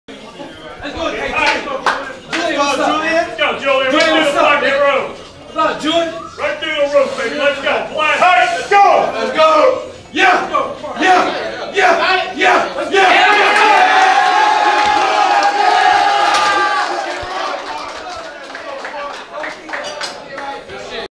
JULIAN HERNANDEZ 325LB BENCH - LIVE HIGH SCHOOL FOOTBALL BROADCAST & LIVE STREAM -
Western High School Wildcats Racking the Weight & Pushing the Iron!